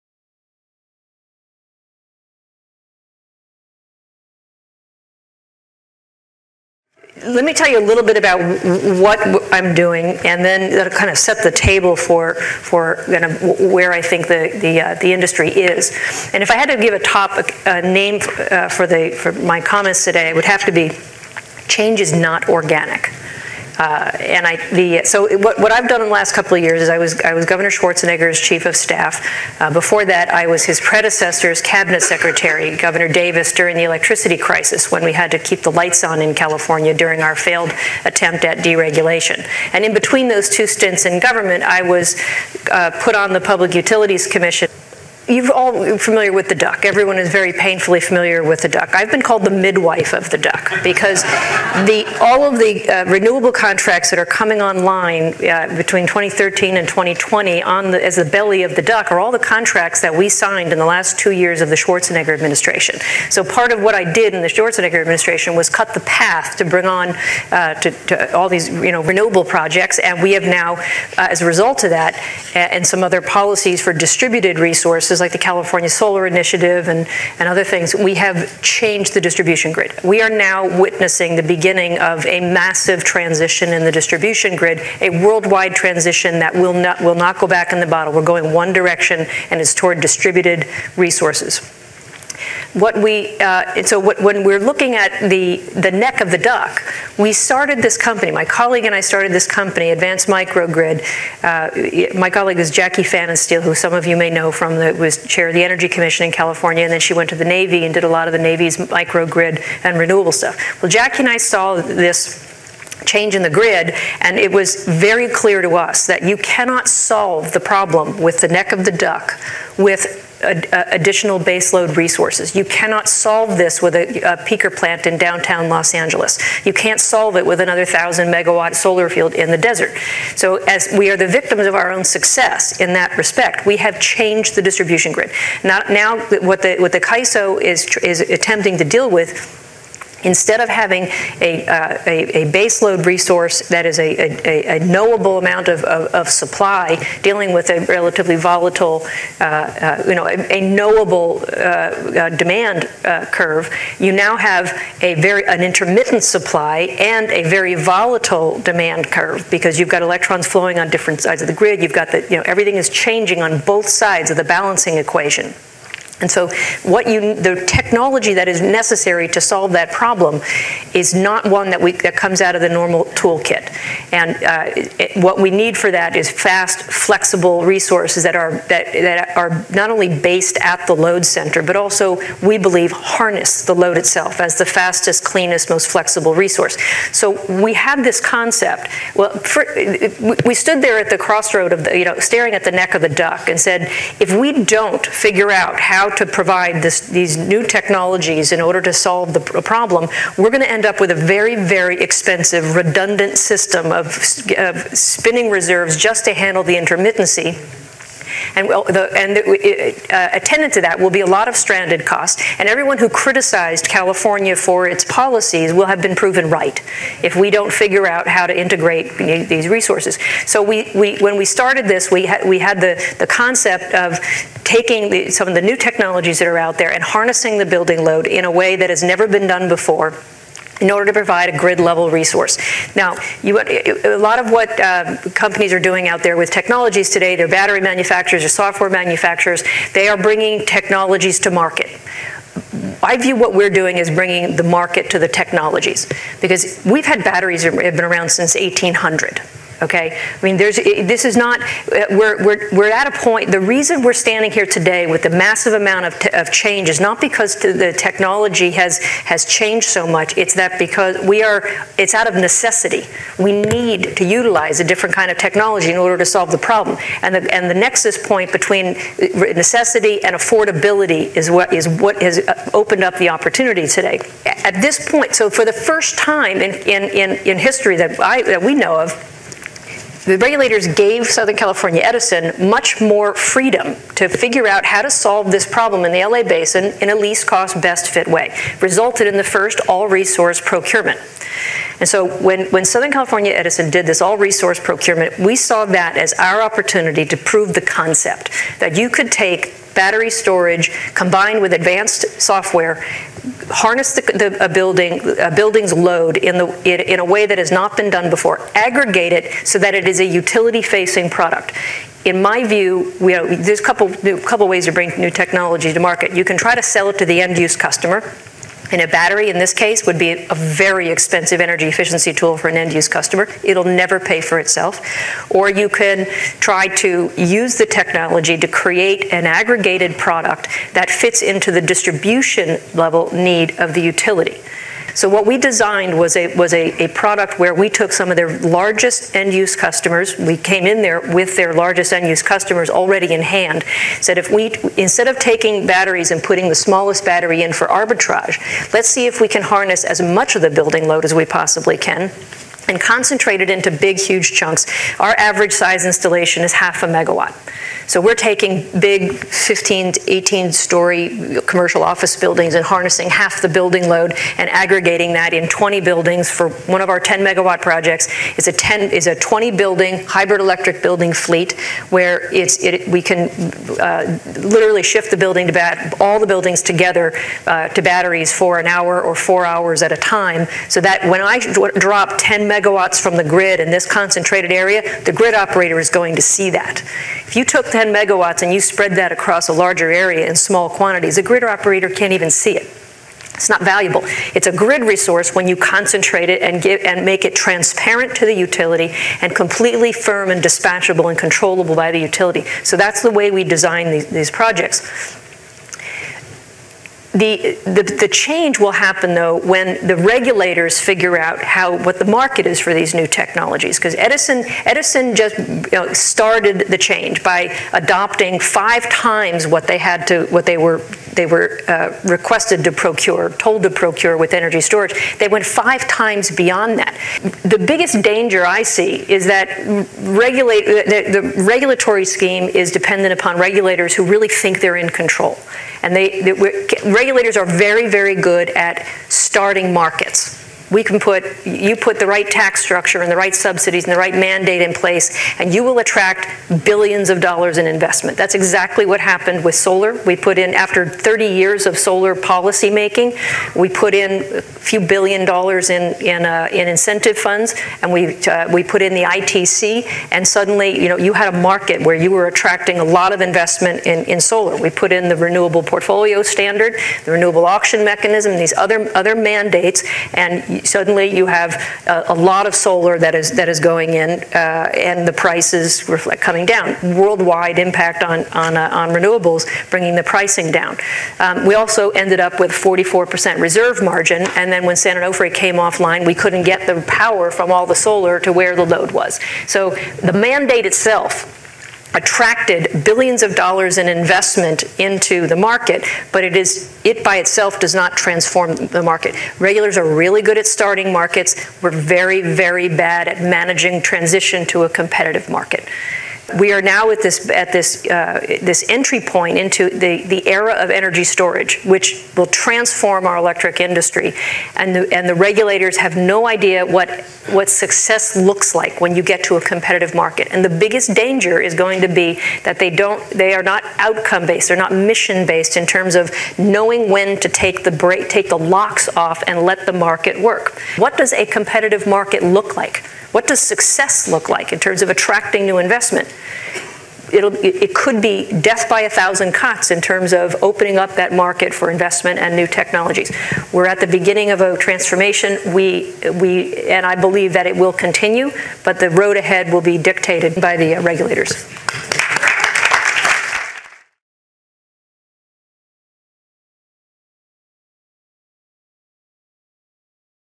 Powering the People 2016: Digital and Distributed Power Grid - Susan Kennedy
On Thursday, March 17, 2016, The Edison Foundation and the Institute for Electric Innovation brought together a group of electric utility and technology company executives, policymakers, regulators, and other thought leaders for an afternoon of dialogue and discussion on key trends driving change in the electric power industry.

In this video Susan Kennedy, Founder and CEO of Advanced Microgrid Solutions, discusses the digital and distributed power grid.